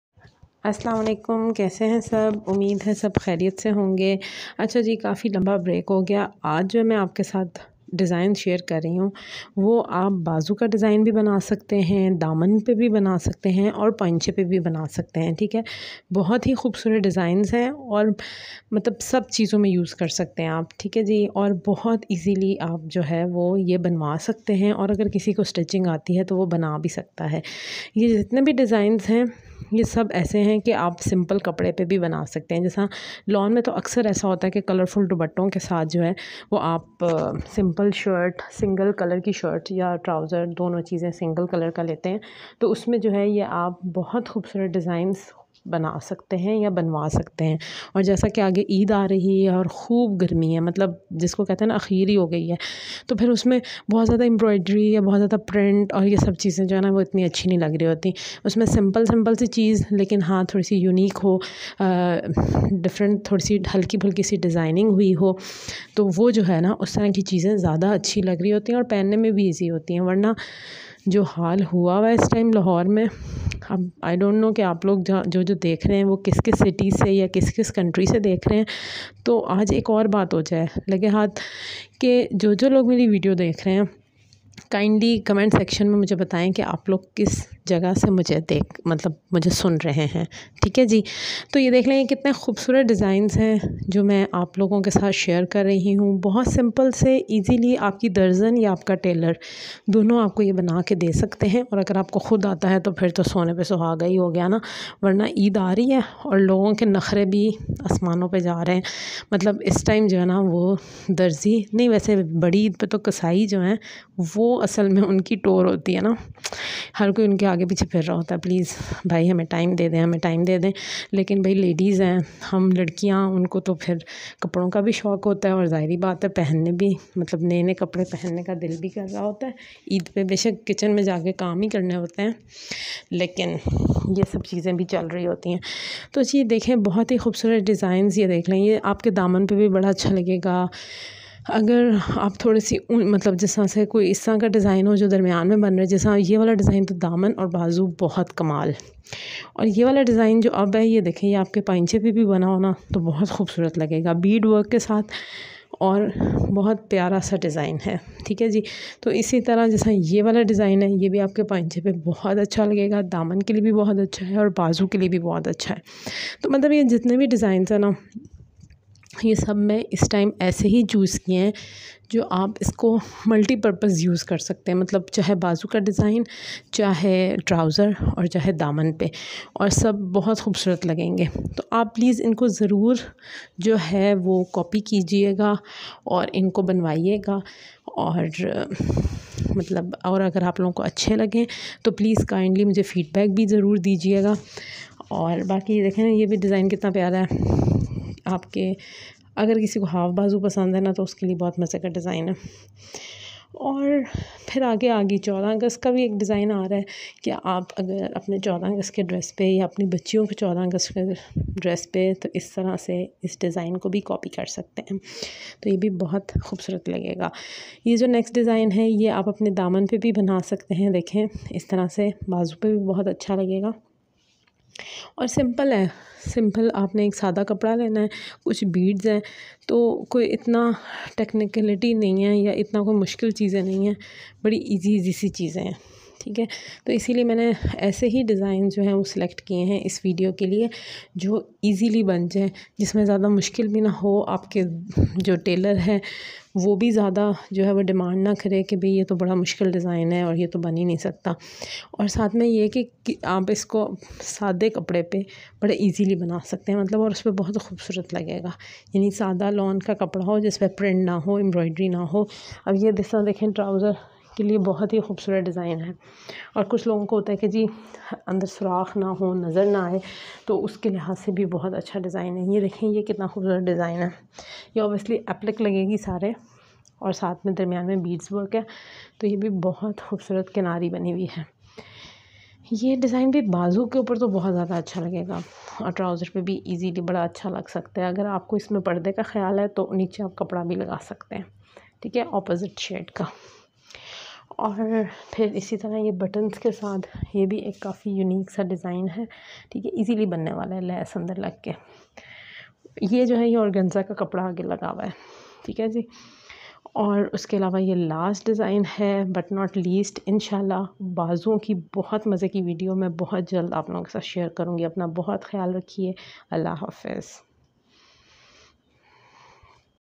Latest design for lawn dresses sleeves بازو daman دامن paincha پائنچہ
Lawn dress design 2021/ latest design / sleevesdesign / damandesign /
in this video very easy to make design for lawn dresses sleeves, daman and paincha are share.
thanks 😊